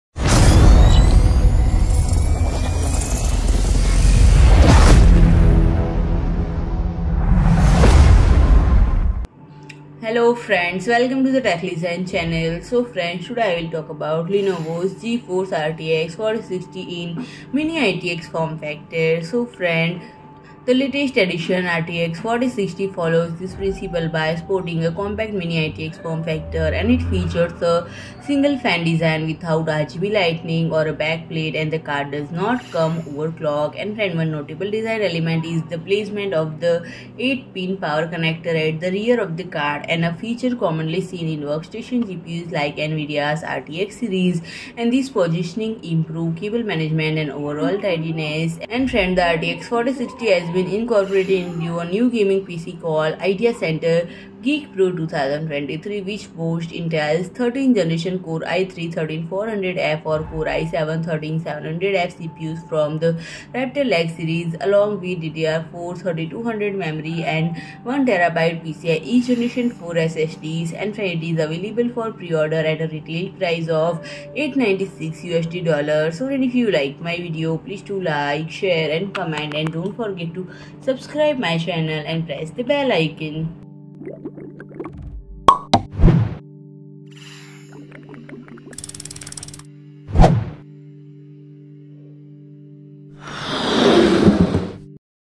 LENOVO GeForce RTX 4060 Mini-ITX Form Factor GPU Launched - Explained All Spec, Features And More
PRODUCT BRAND STORE AMAZON

Lenovo adheres to a minimalist aesthetic when introducing new cards in the RTX 40 lineup. The latest addition, the RTX 4060, follows this principle by sporting a compact Mini-ITX form factor. It features a single-fan design without RGB lighting or a backplate. Notably, the card does not come overclocked. One notable design element is the placement of the 8-pin power connector at the rear of the card, a feature commonly seen in workstation GPUs like NVIDIA’s RTX series. This positioning improves cable management and overall tidiness. It’s important to mention that Lenovo does not directly sell graphics cards. The only way to acquire this model is by purchasing a complete system. The RTX 4060 has been incorporated into a new gaming PC called IdeaCenter GeekPro 2023, which boasts Intel’s 13th Gen Core i3-13400F or Core i7-13700F CPUs from the Raptor Lake series, along with DDR4-3200 memory and 1TB PCIe Gen4 SSDs. The GeekPro system is available for pre-order at a retail price of 6399 RMB (896 USD). Initial shipments are scheduled to begin on July 25th. It’s worth noting that currently, the system is only listed on a Chinese e-tail platform, and it remains unclear whether Lenovo has plans to introduce it to the global market.